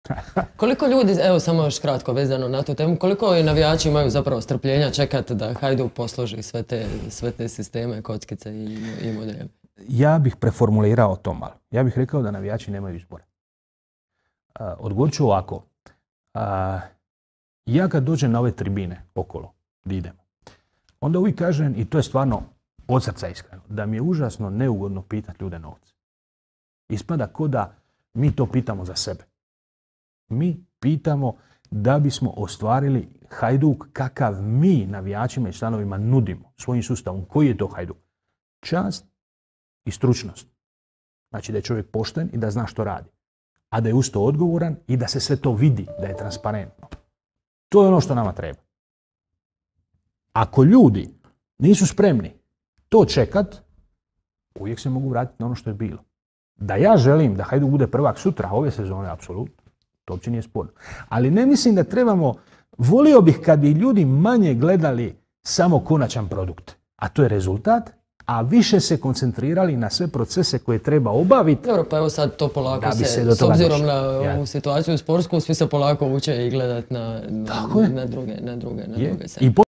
[h+] Ivan Rilov o tome kad će Hajduk biti dobar
Koji je to Hajduk? Čast i stručnost. Da je čovjek pošten i da zna što radi. Da je uz to odgovoran i da se sve to vidi, da je transparentno. To je ono što nama treba.